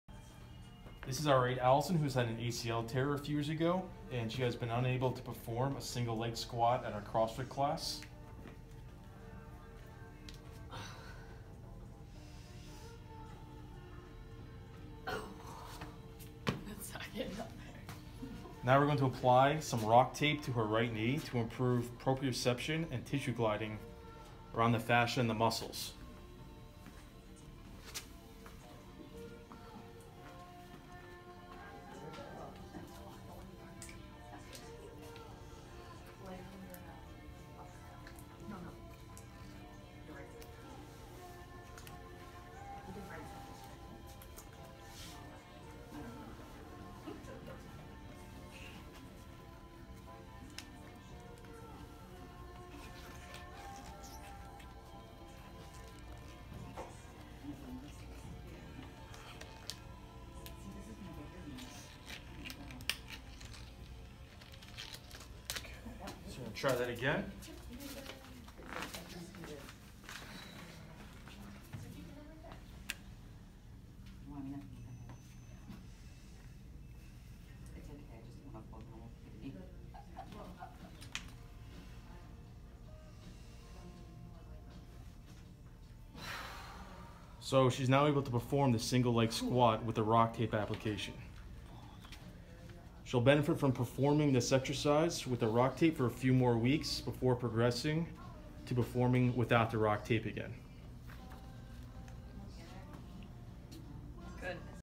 Single Leg Squat on Bosu with RockTape RockFloss
Our aide Alison demonstrates a single leg squat on a Bosu ball. After being unable to complete the movement on her own, once RockFloss is applied, she is able to complete the squat. This improves the proprioception and tissue gliding between her fascia and muscles.